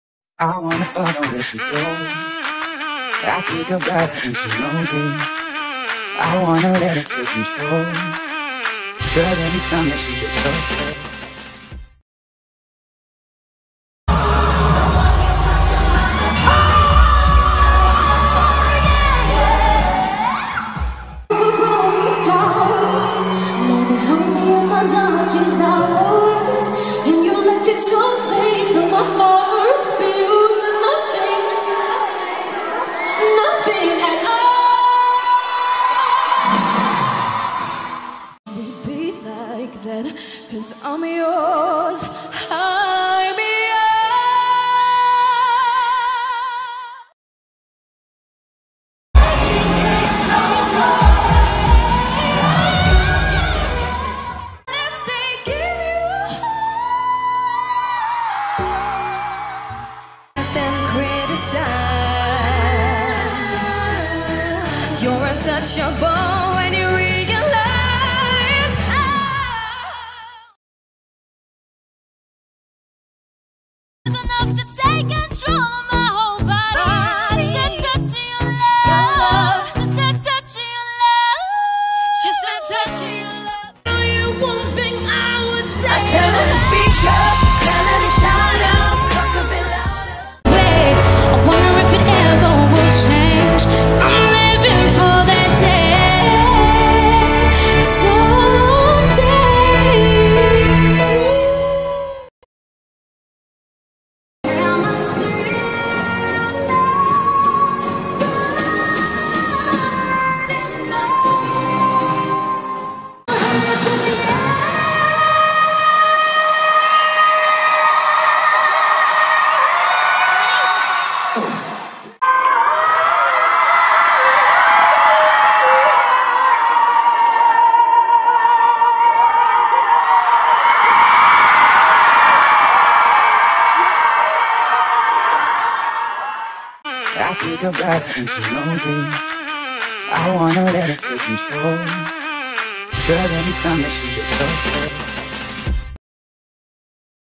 Little Mix | BEST HIGH NOTES
Hi guys,
in this video I give you some of Little Mix's best high note. Sorry for the bad quality, my computer crashed so I had to edit on my phone.